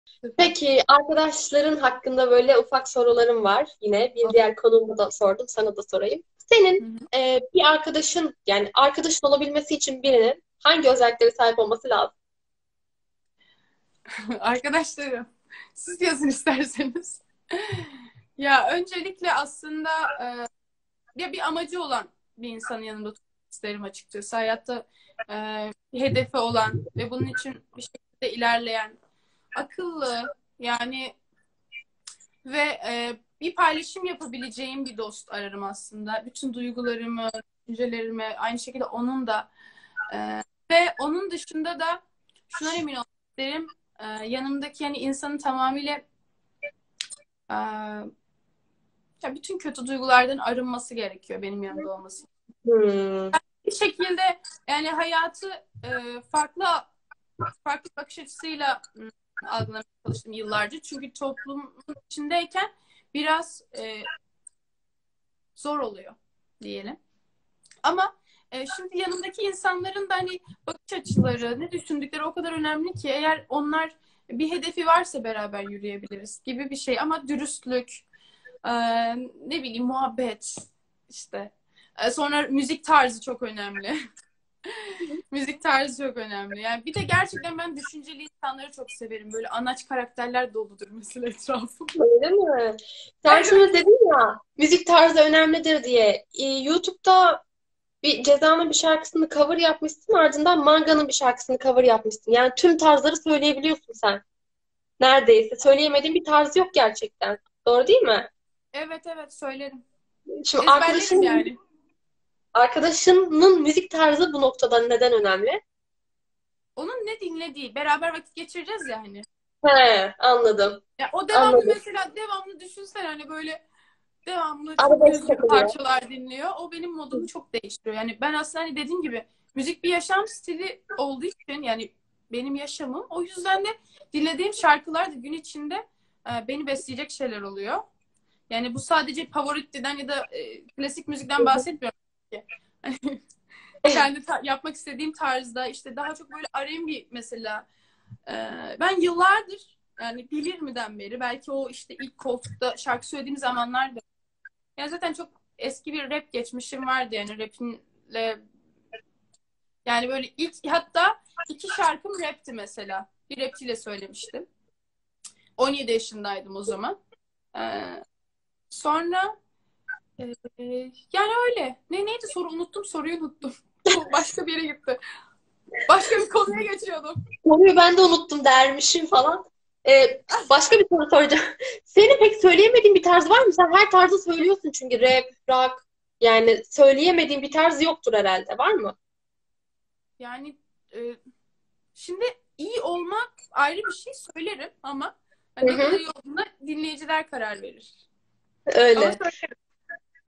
Feride Hilal Akın İle Arkadaş Olmak - Buse Özübek Online Soru Cevap
Buse Özübek ile instagram sohbetine sevilen sanatçı Feride Hilal Akın konuk oldu.
Siz de milyon tv instagram hesabını takip edin ve sevdiğiniz sanatçılara soru sorma imkanını yakalayın!